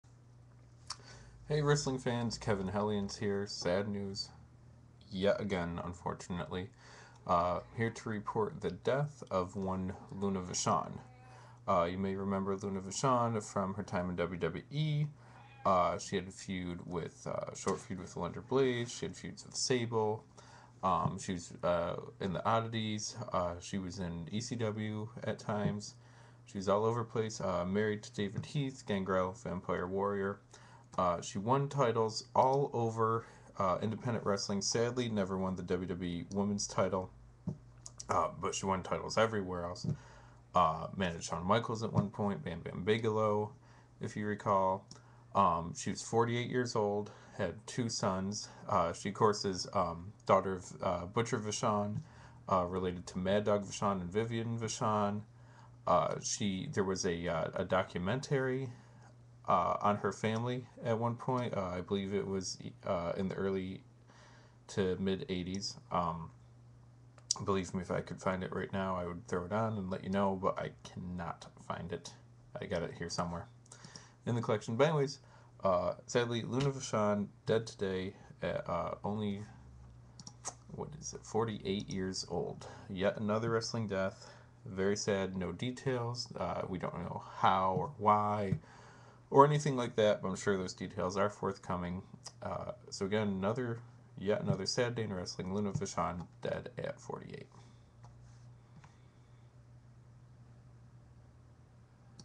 WWE Alumni Luna Vachon dead at 48.
Former WWE Superstar and Diva Luna Vachon died today at 48 years old.  Details are still forthcoming.